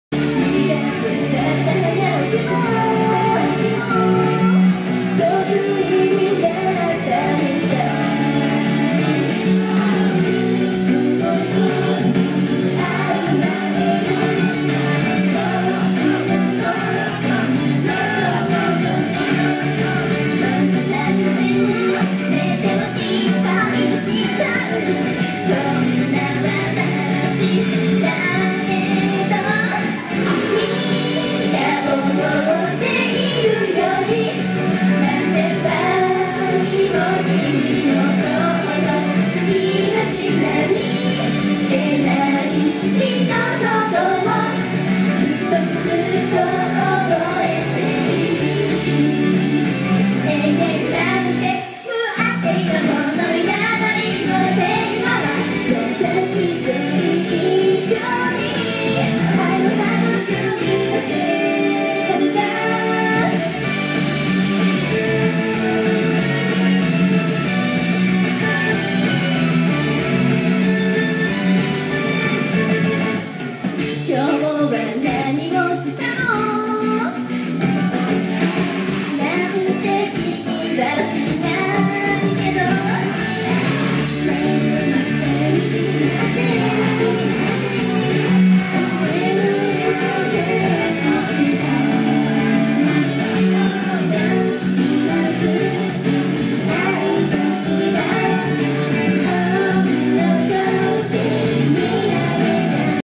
メイビー ME ヨコハマカワイイパークLIVE
メイビーMEのヨコハマカワイイパークで
野外イベントLIVEを撮影した動画です。